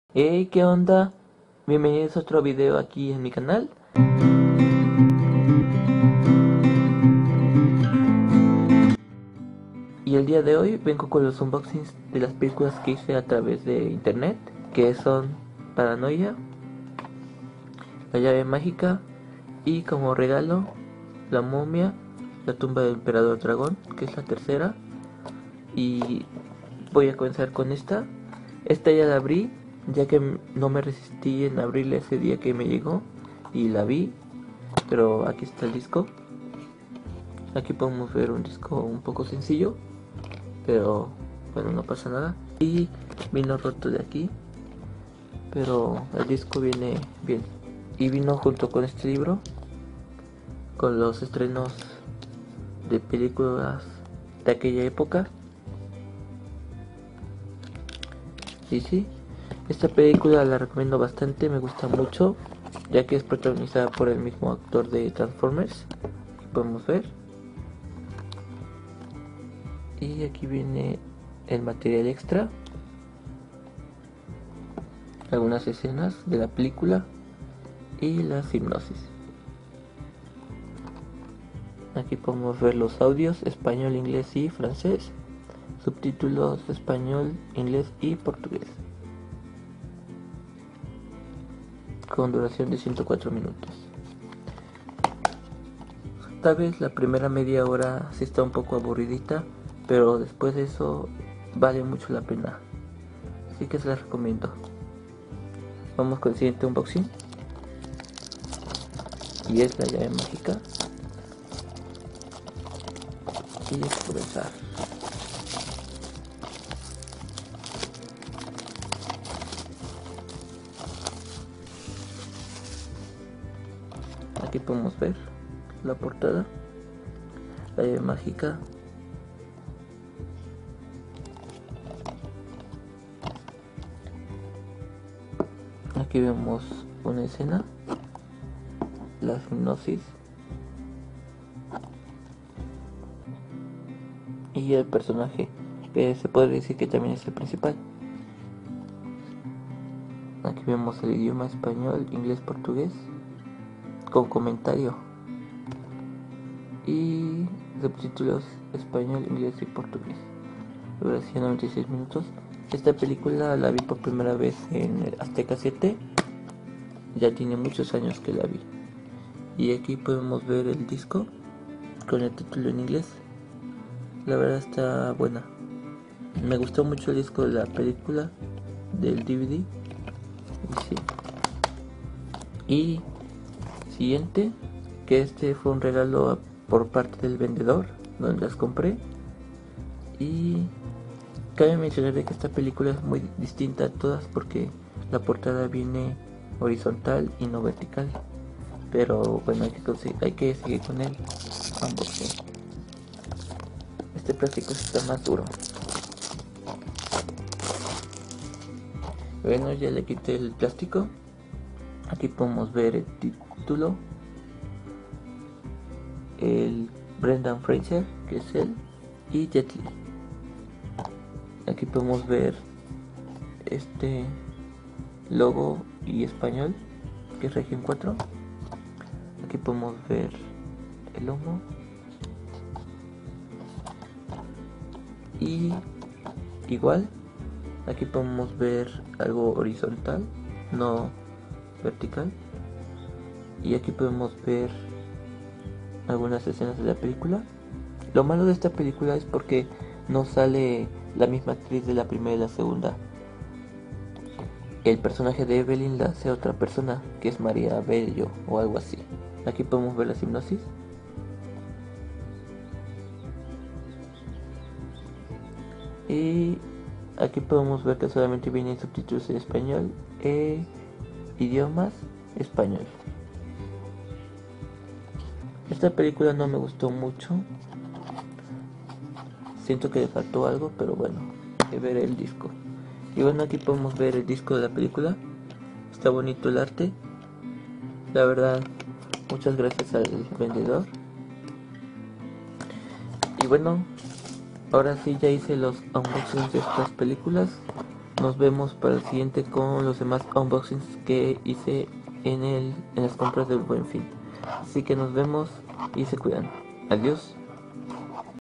Unboxing Paranoia, La llave mágica y La momia la tumba del emperador dragón DVD
Y bueno, hoy les traigo los unboxing de las películas Paranoia (Disturbia), La llave mágica y La Momia la tumba del emperador dragón que esta última fue regalo y no sabía jaja, bueno espero que les guste el vídeo :D